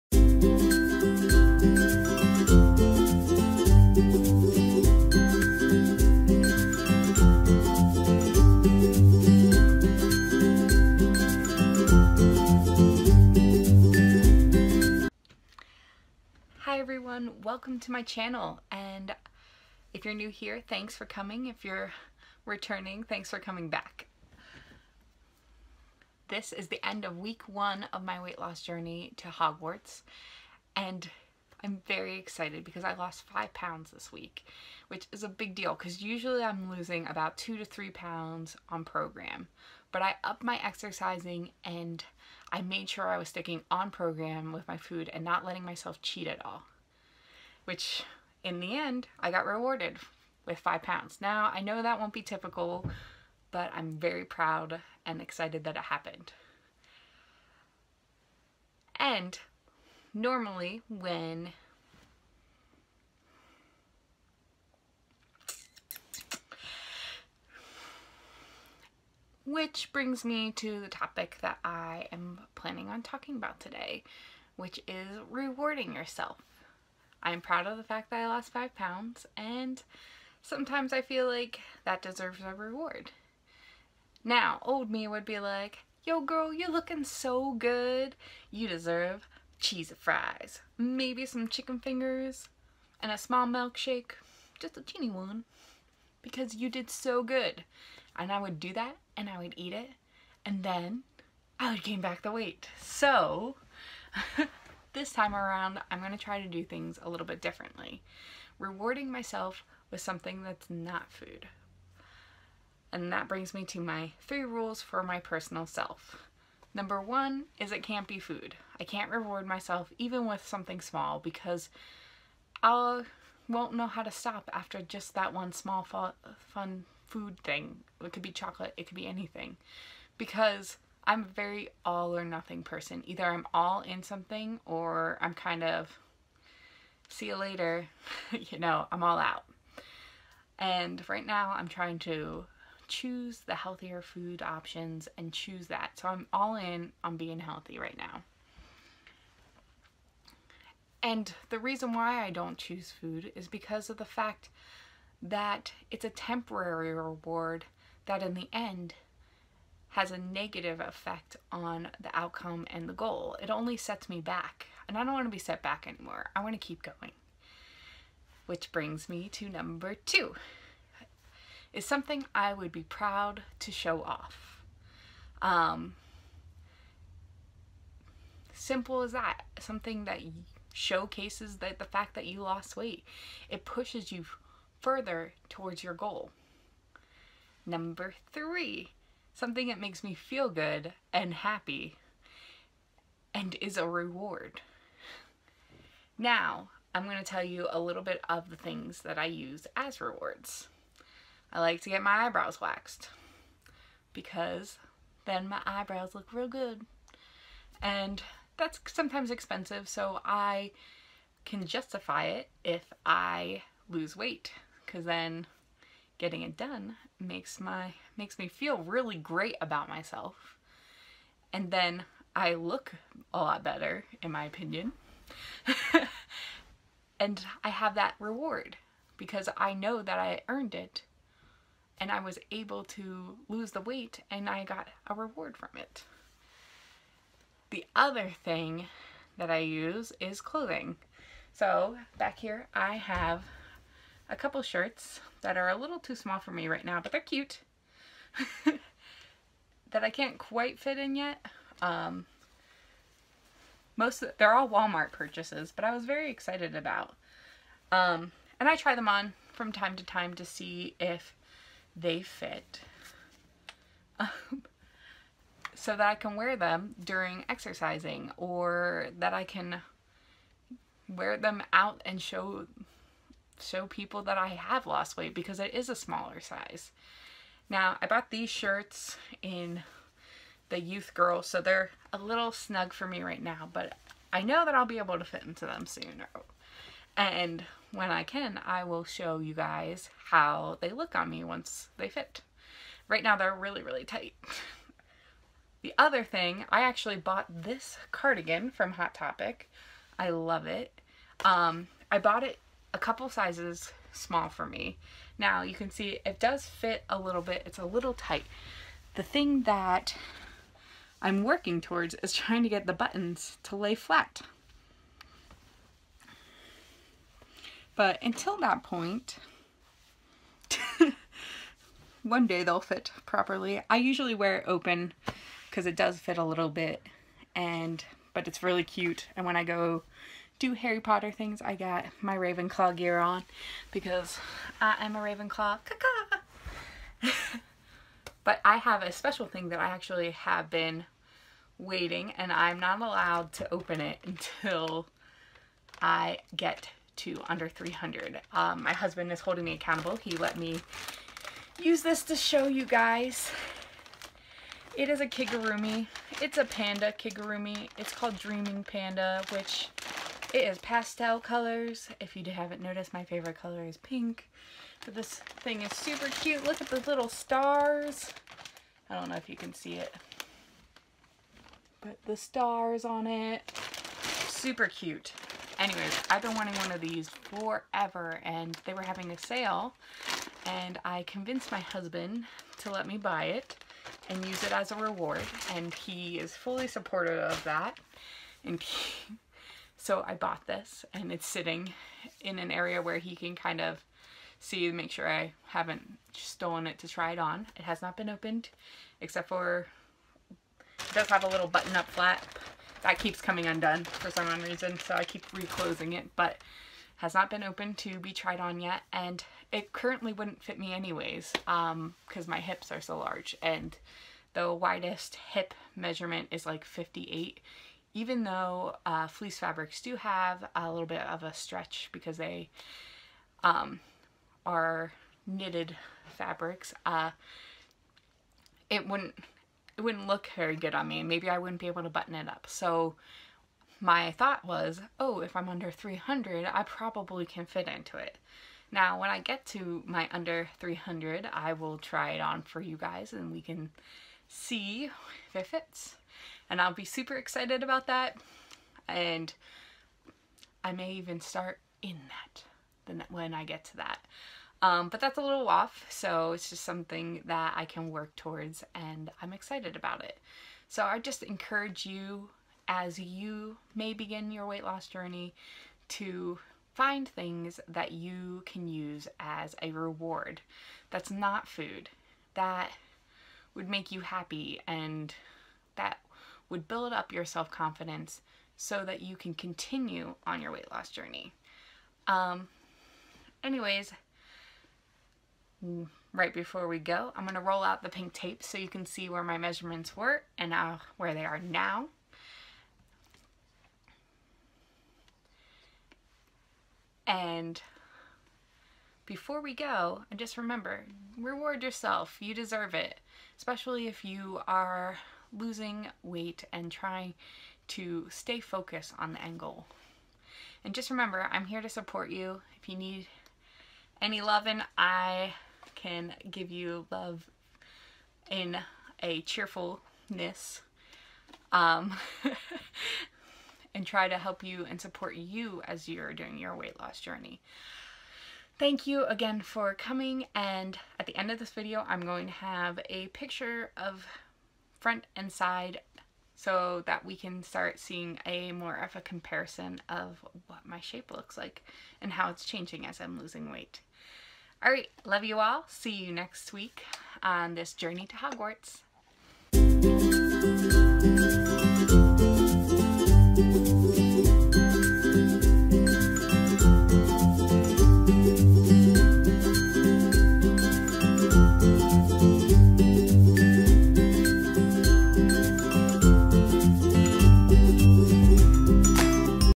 Vlog# 2 End of Week One too Reward or Not to Reward
This was filmed on 5/7/2017
Thanks for joining me on my weight loss journey to Hogwarts this is the end of week one and I am talking about my process of how I am going to reward myself to help keep myself motivated and to keep working hard.
 I apologize I am still new at this and learning...I promise that I am just as awkward in real life but many find that endearing about me.